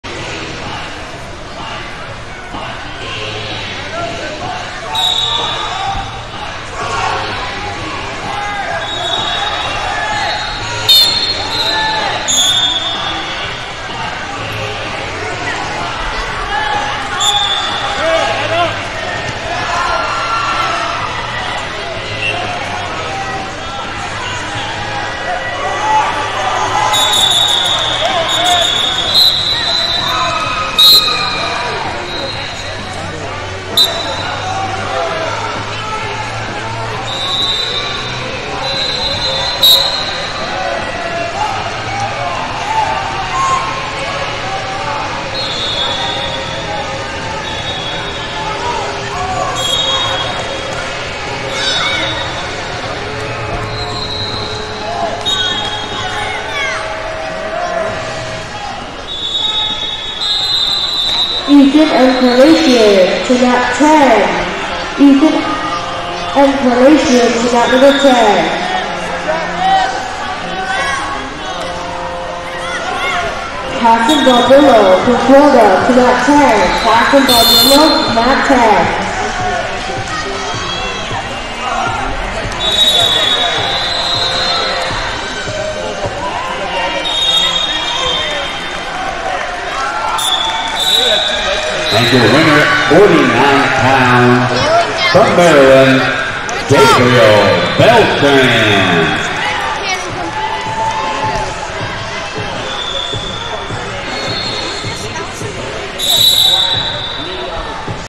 63 Lbs Semifinal - Ryder Yates, UT Vs Kamden Gallus, MN 59ab
2023 Kids Freestyle & Greco-Roman National  Champs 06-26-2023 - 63 Lbs Semifinal - Ryder Yates, UT Vs Kamden Gallus, MN 59ab